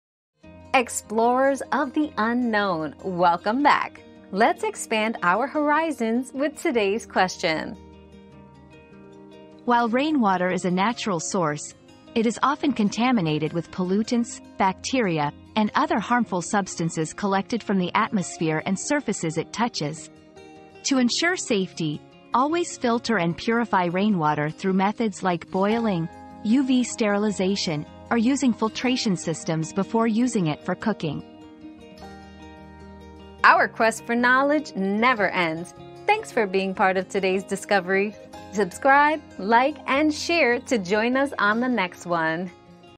Is it safe to use rainwater for cooking?
Rainwater Cooking Safety 👉 Cooking with Rainwater 👉 Learn how to safely use rainwater for cooking by filtering and purifying it before consumption. Stay healthy and avoid contaminants with these simple methods.

Laura S. Harris (2024, July 4.) Is it safe to use rainwater for cooking?
    🌐 AskAbout.video